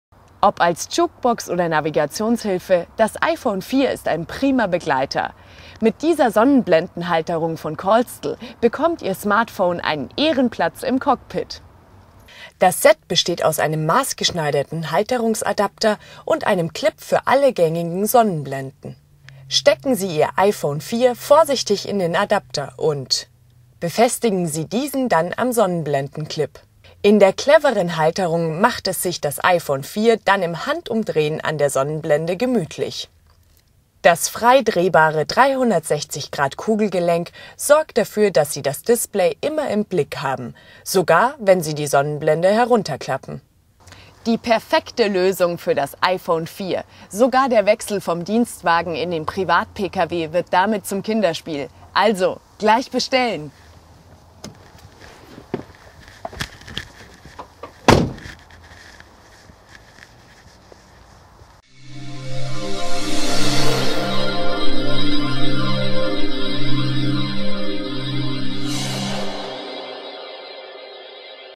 Callstel Praktische Sonnenblenden-Halterung 360° für iPhone 4 & 4S
Die montagefreie und maßgeschneiderte Halterung passt in jedes Auto

Die schnelle Lösung für Ihr iPhone4: Diese passgenaue 360°-Halterung wird einfach an die Sonnenblende geklemmt. So stört sie nicht Ihre Sicht und macht jeden Wagenwechsel schnell mit.

Dank voll drehbarem Kugelgelenk haben Sie das Display immer perfekt im Blick. Auch wenn Sie die Sonnenblende herunterklappen.

    Montagefreie Kfz-Halterung maßgeschneidert für iPhone 4 & 4S
    Sofort startklar: Fester Clip für alle gängigen Sonnenblenden
    Immer optimal im Blick: Frei drehbares 360° Kugelgelenk
    Aufstecksystem: Gerätehalterung lässt sich einfach abnehmen